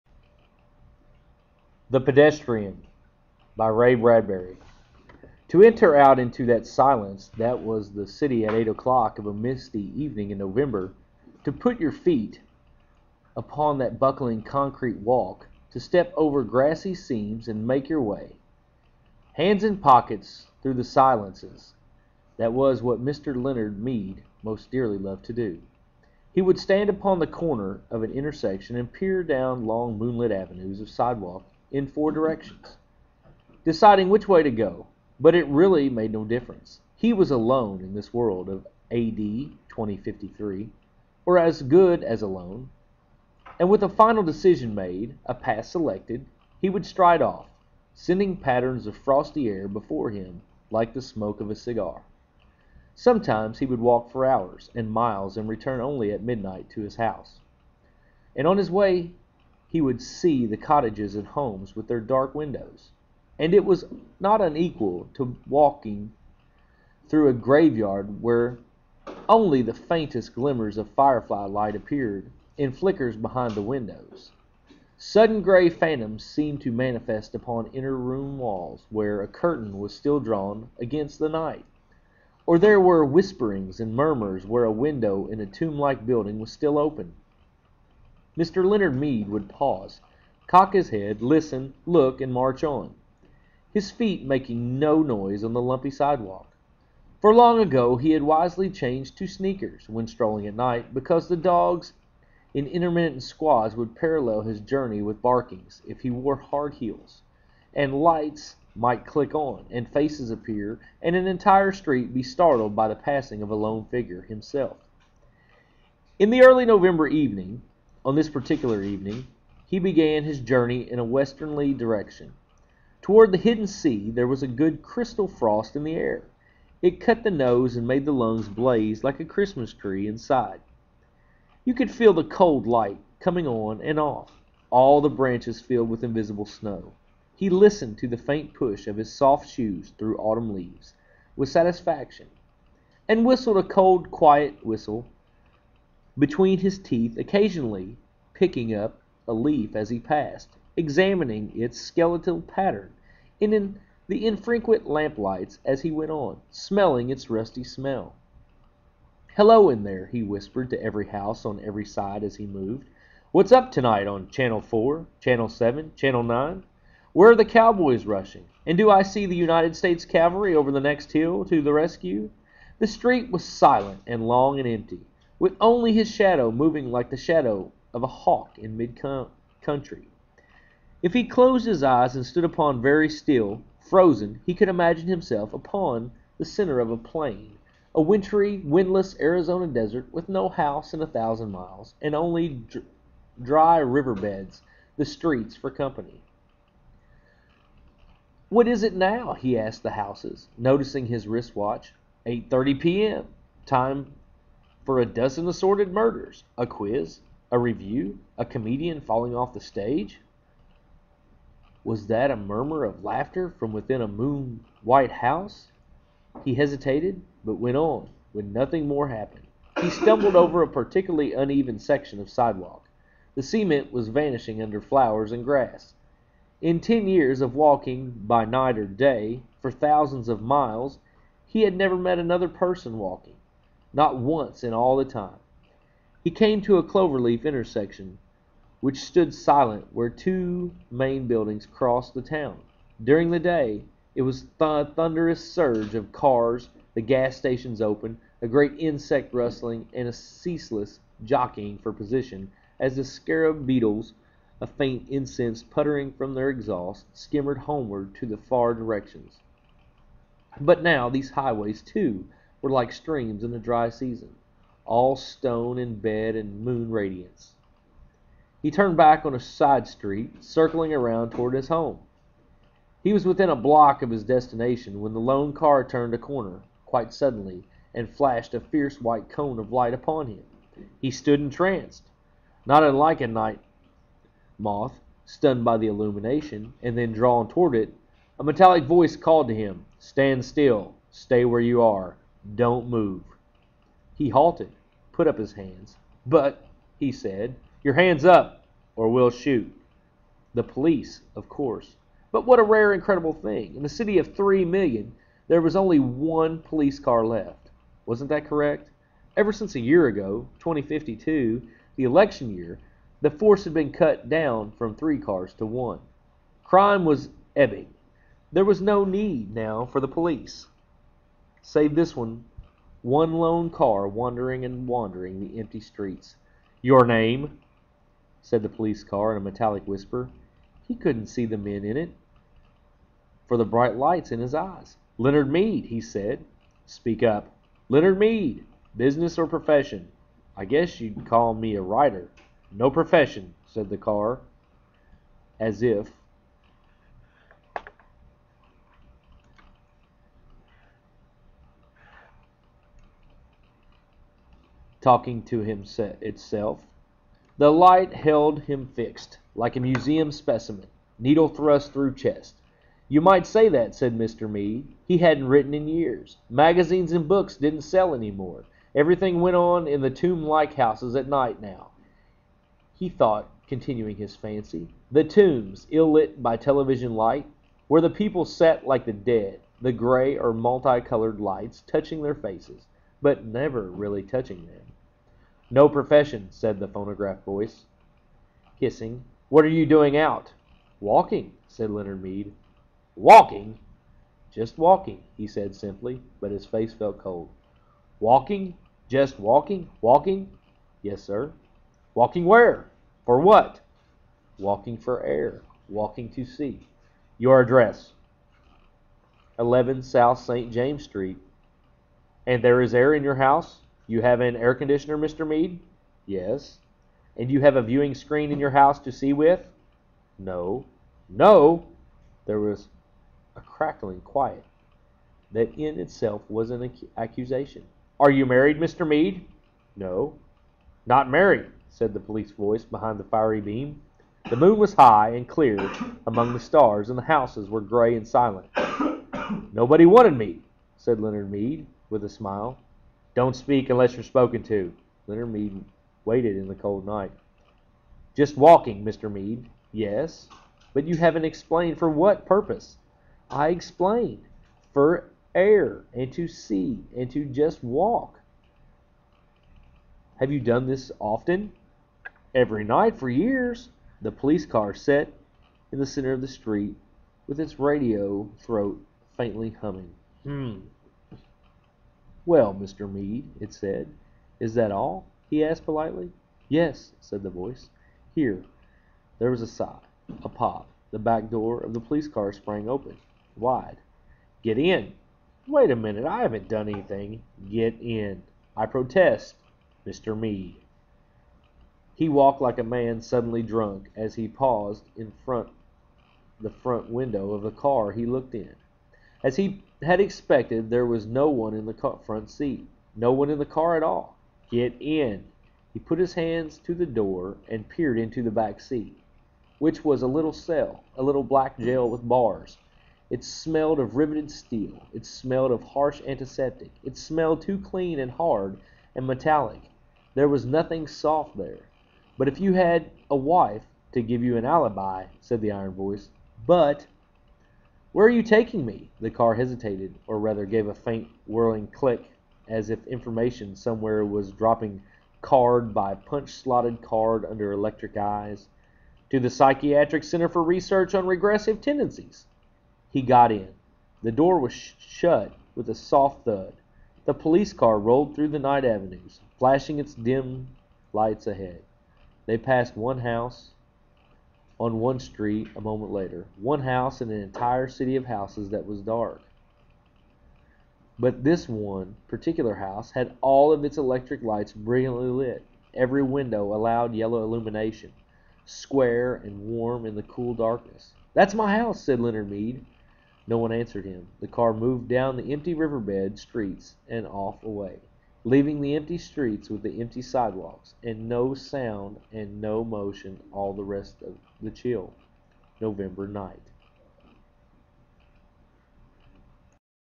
The Pedestrian by Ray Bradburry.avi
This is the reading of Bradburry's 1951Short Story "The Pedestrian". Read by Chris Stockdale